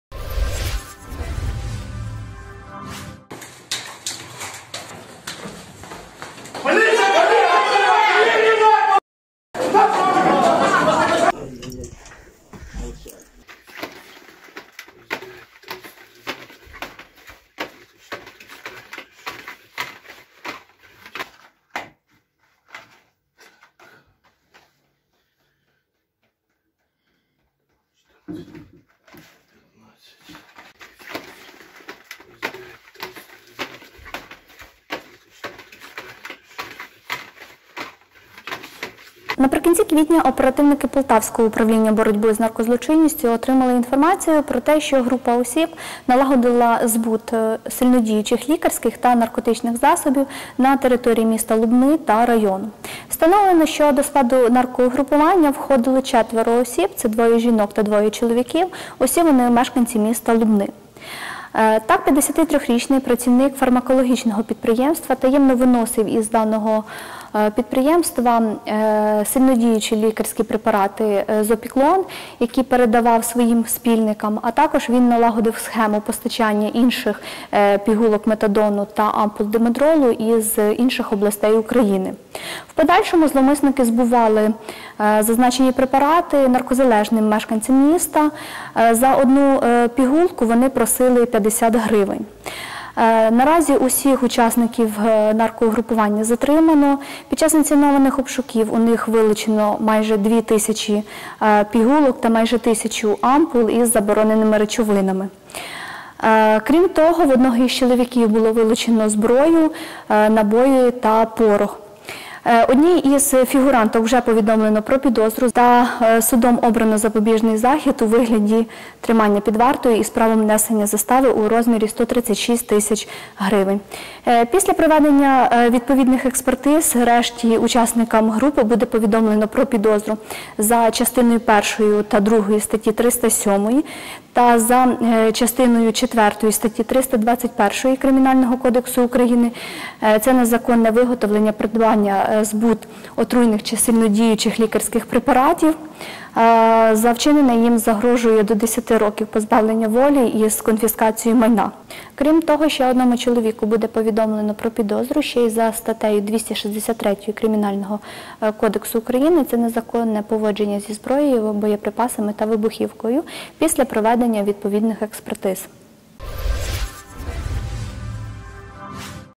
У Лубнах викрили групу наркодилерів
В Лубнах поліцейські викрили групу наркодилерів у незаконному збуті сильнодіючих лікарських препаратів

Правоохоронці встановили, що до протиправної діяльності причетні четверо мешканців міста Лубни – двоє жінок та двоє чоловіків.